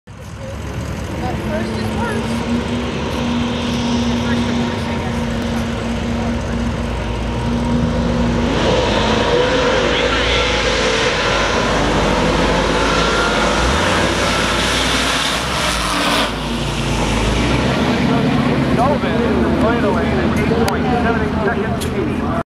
Sullivan
Mustang takes the win at Irwindale summit sportsman july '07